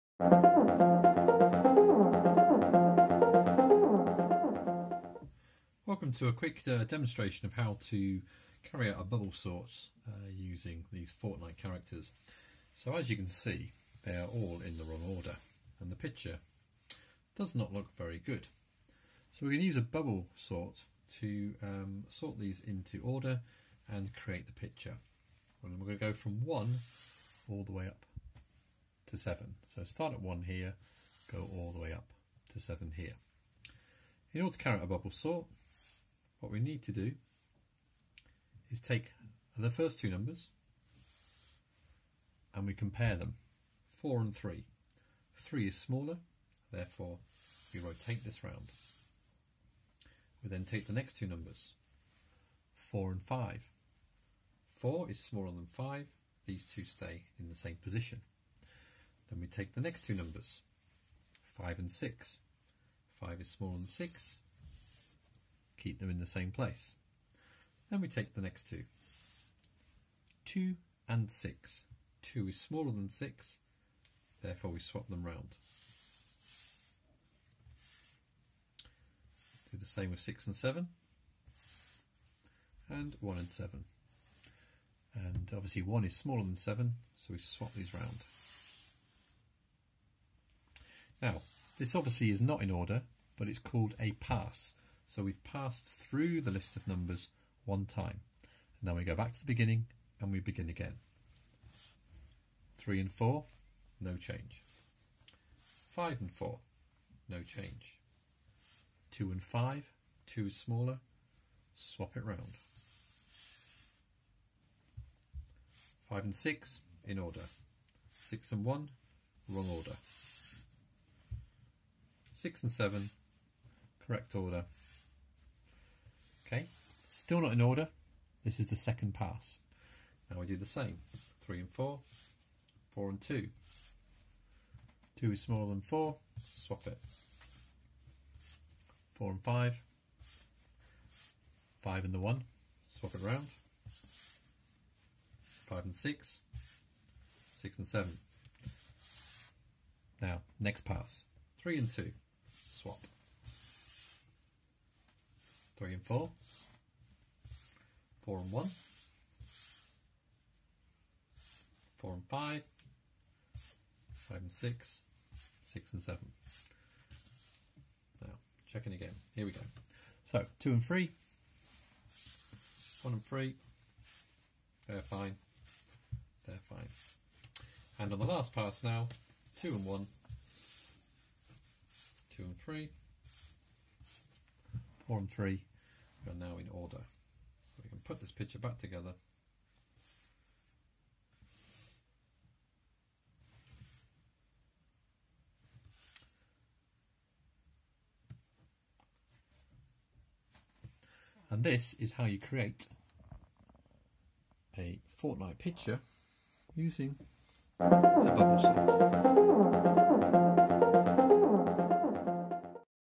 GCSE Computing Bubble Sort - Fortnite
An example of a Bubble Sort:
Sorting Fortnite characters into order, with a Bubble sort.